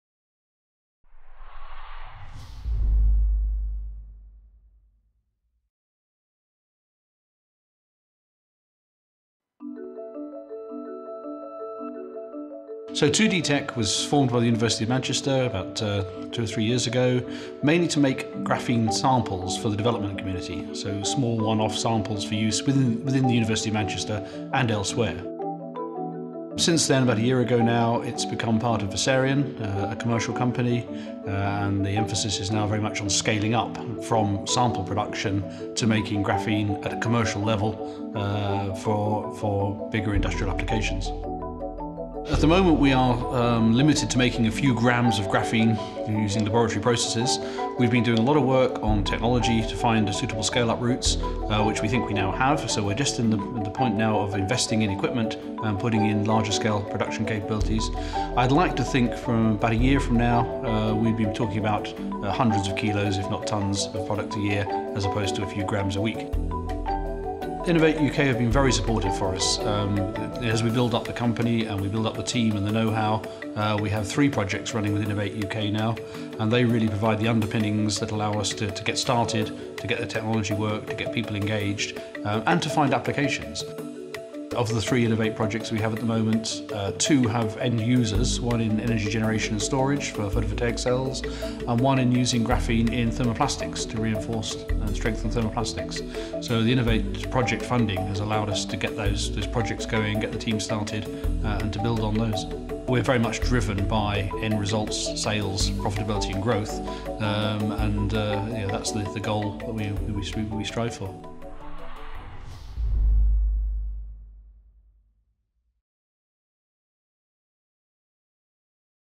Innovate UK success story: 2-D tech unlock graphene's potential (ISCF - Clean Growth)
A university spin-out is scaling up its production of the high-strength material graphene to commercial levels with support from Innovate UK

Graphene is a new material that is extremely thin and extremely strong. It has many potential applications, but progress on applying it commercially has generally been slow.

Since its acquisition by rapidly growing materials business Versarien a year ago, 2-DTech has focused on scaling up from sample production to making graphene at a commercial level for bigger industrial applications.

2-DTech believes it has refined the technology for scaling up production and is now investing in the equipment that will allow it to produce graphene at a commercial level.